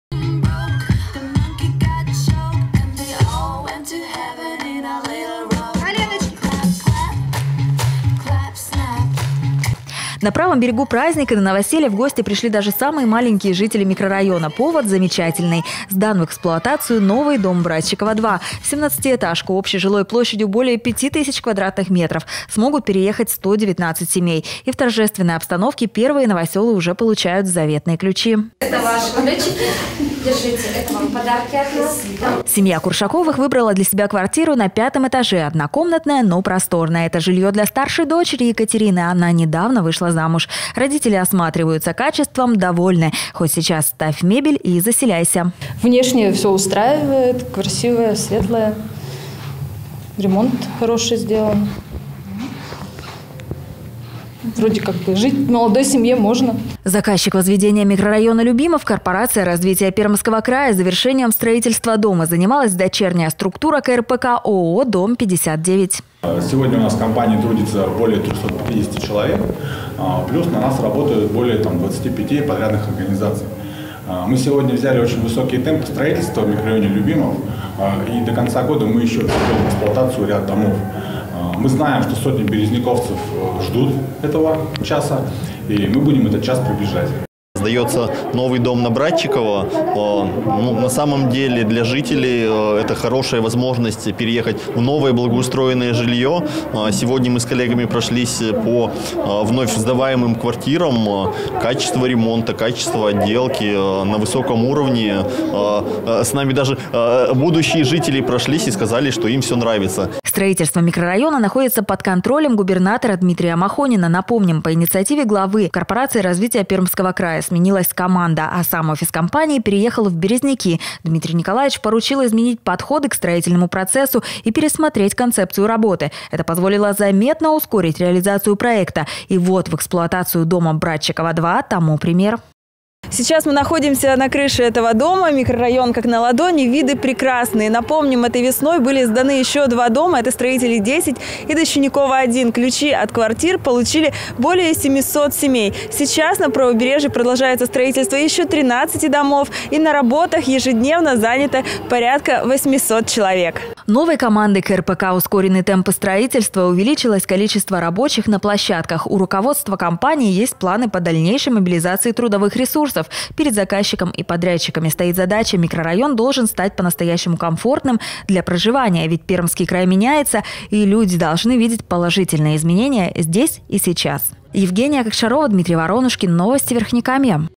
Сдан дом в "Любимове"
А вот в "Любимове", слава Богу, радостные события! Там в эти выходные сдали в эксплуатацию 17-этажный дом на Братчикова, 2. По новеньким квартирам вместе с счастливыми жильцами прогулялась Евгения Кокшарова.